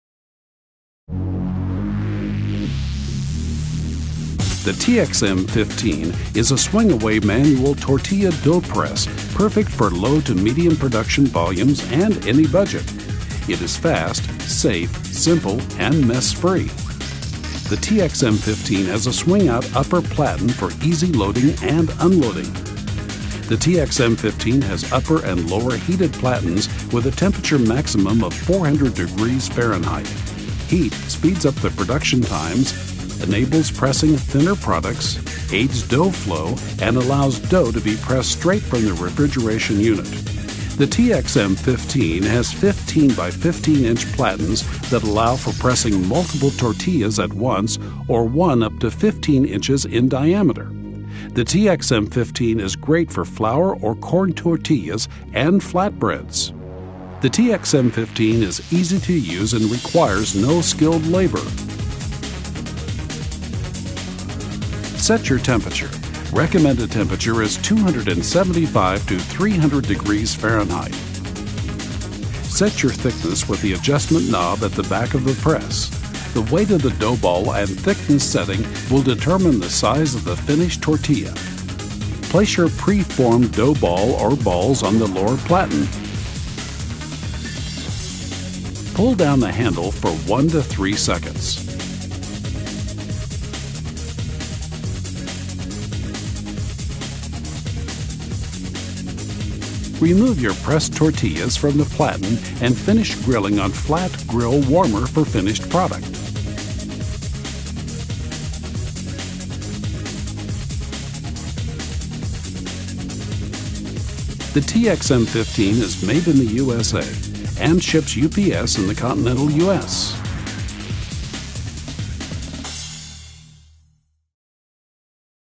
doughXpress® - Economy Manual Tortilla Press (TXM-15)
The doughXpress® economy tortilla presses are designed for low to medium production volumes. These manual tortilla presses easily increase production with their simple easy construction. Both are great for either flour or corn tortillas and flat breads. This equipment pairs well with our tortilla warmers for finishing.

The TXM-15 features 15”x15” platens capable of making 4 tortillas at once. The TXM-15 is a swing away design with upper and lower heated platens.

00:00:00 Intro
00:00:04 TXM-15
00:01:03 Demo
00:01:54 Exit